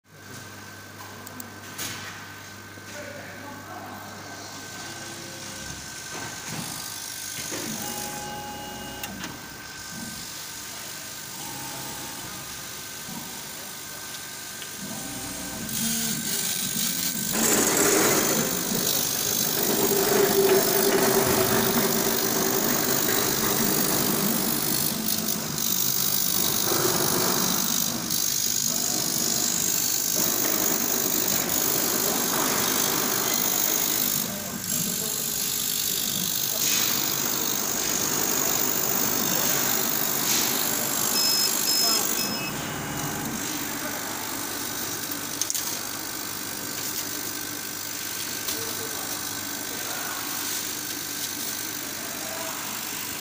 CO2 Laser Cutting Machine with CCD - Cutting Paper
⇲ The machine adopts the strip shape platform, which can bear heavier materials such as organic glass, wood, and so on.
⇲ Original Singapore lens with molybdenum base, making the equipment produce smaller facula and stronger cutting force, greatly improving working efficiency and reducing cost.
⇲ Exhausting, absorbing and assistant blowing systems solve the problem that troubles the laser equipments manufacturing industry for years, extending the life of the laser tube.